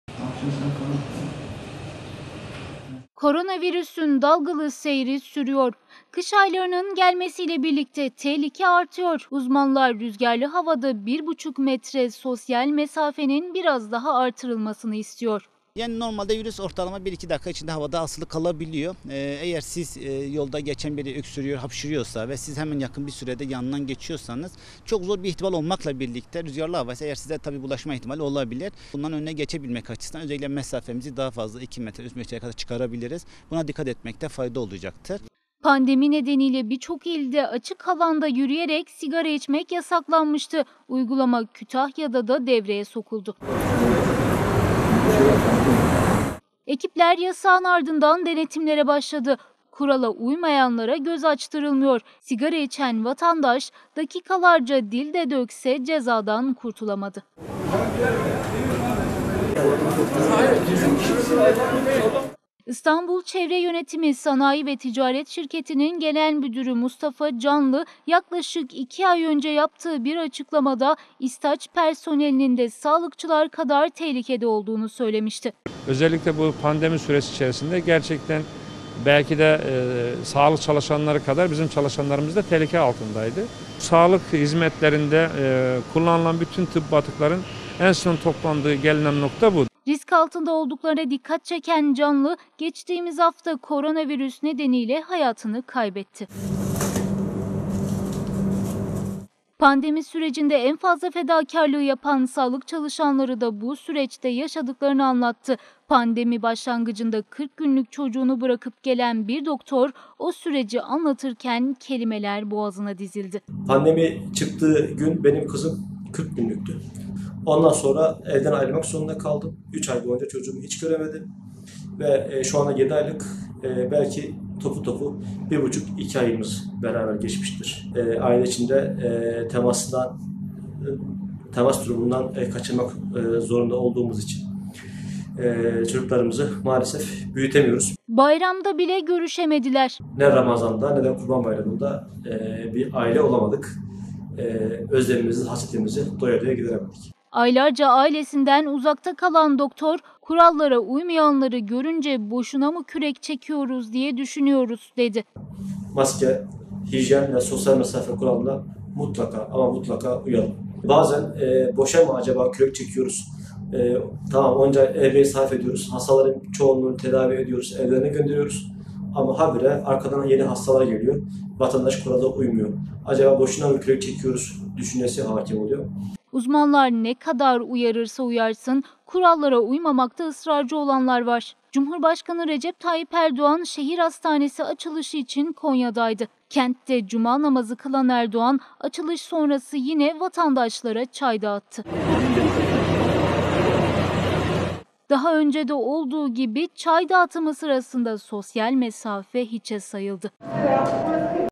Sosyal mesafeye rüzgar ayarı!
Kış aylarının gelmesiyle beraber tehlike artıyor... Uzmanlar rüzgarlı havada 1.5 metre sosyal mesafenin biraz daha arttırılmasını istiyor...

TV5 uydu bilgileri:
TÜRKSAT 3A/4A
Frekans: 12729
Sembol: 30000
Polarizasyon: V (Dikey)